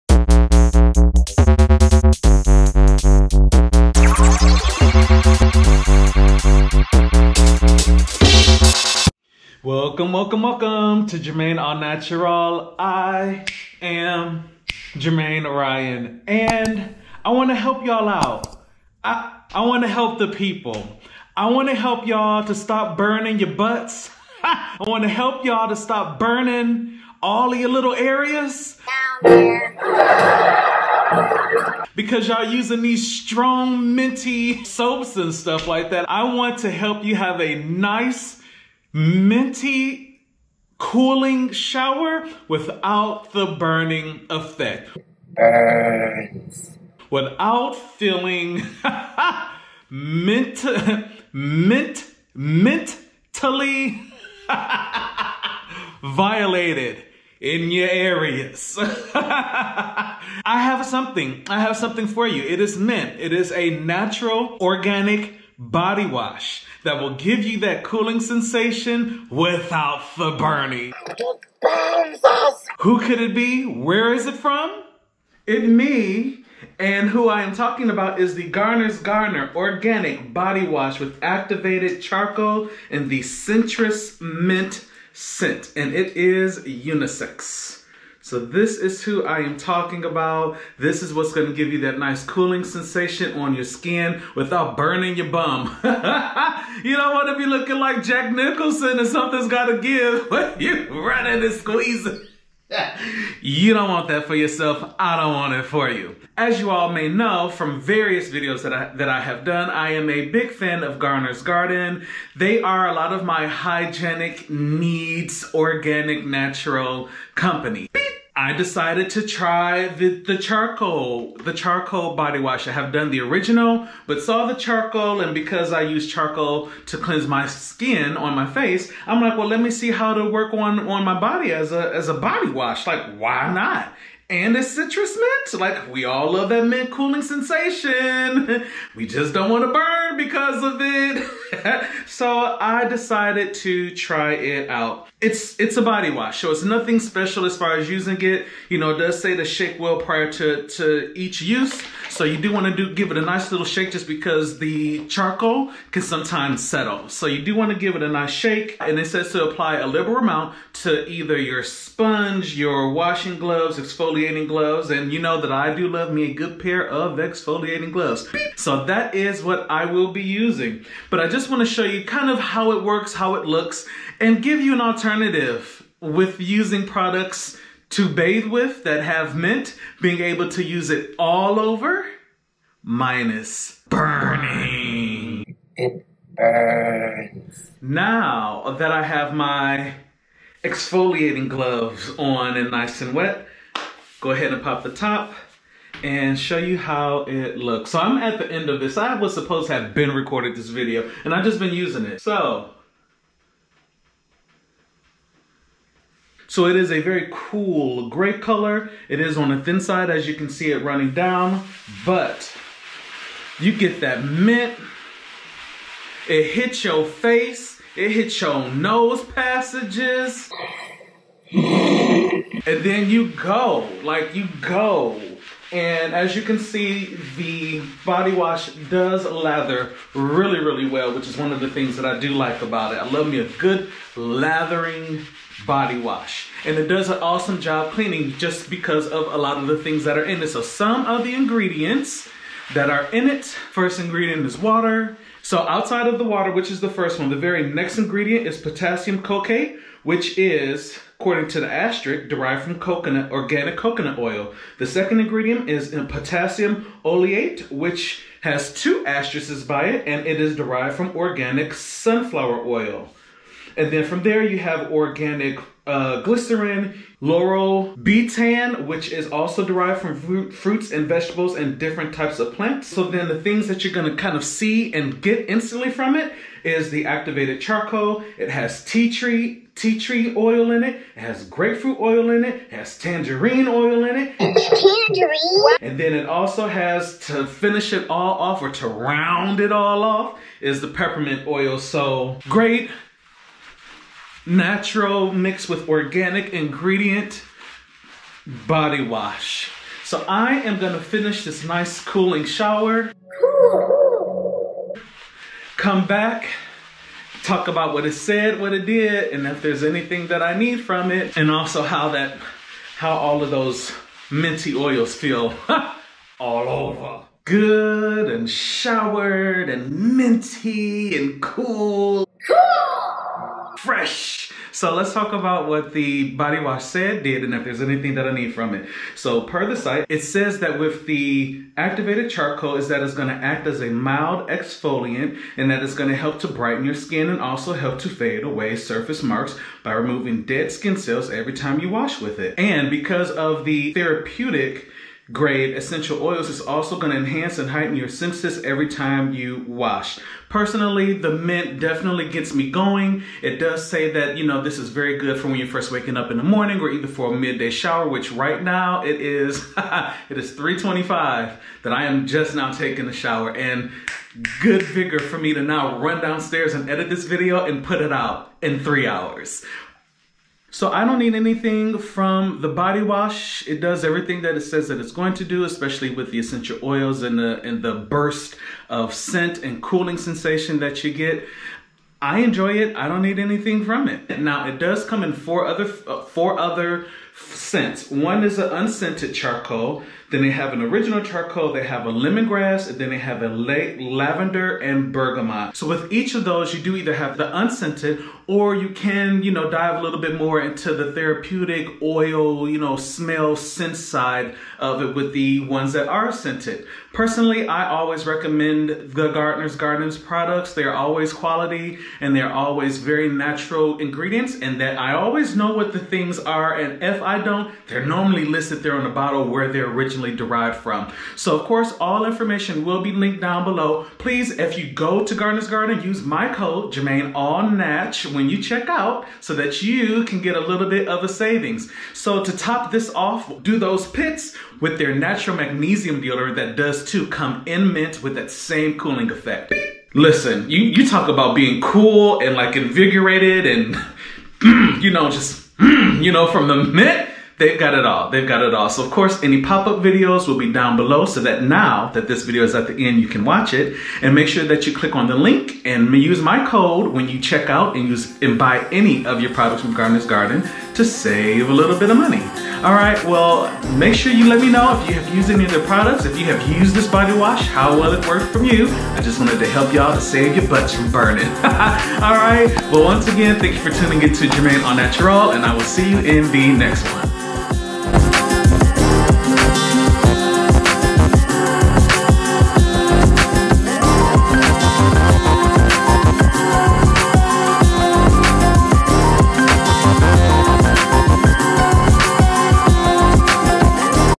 Garners Garden Activated Charcoal Organic Body Wash in Citrus Mint
You can use peppermint body wash and not burn ya butt!! With the right amount of natural essential oil you can get the cooling sensation minus the burn.
I tries the Garners Garden Activated Charcoal Organic Body Wash and was able to avoid sizzling down there.

Thank you for visiting JermaineAuNatural☺️